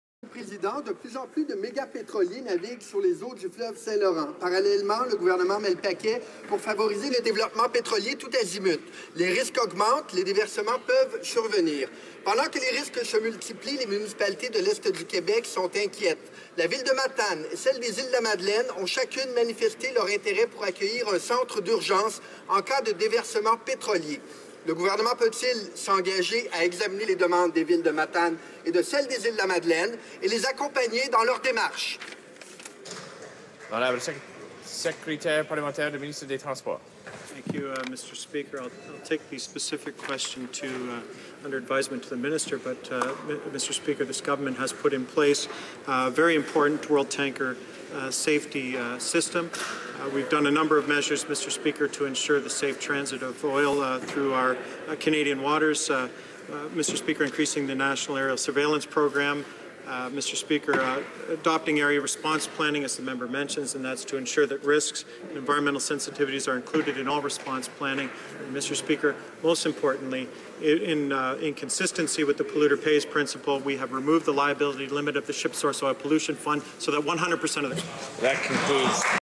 Un centre d'urgence en cas de déversement pétrolier est demandé !
Monsieur le Président, de plus en plus de méga-pétroliers naviguent sur les eaux du fleuve Saint-Laurent. Parallèlement, le gouvernement met le paquet pour favoriser le développement pétroliers tous azimuts. Les risques augmentent et les déversements peuvent survenir. Pendant que les risques se multiplient, les municipalités de l'Est du Québec sont inquiètes.
    La ville de Matane et celles des Îles-de-la-Madeleine ont chacune manifesté leur intérêt pour accueillir un centre d'urgence en cas de déversement pétrolier. Le gouvernement peut-il s'engager à examiner les demandes de la ville de Matane et de celles des Îles-de-la-Madeleine et de les accompagner dans leurs démarches?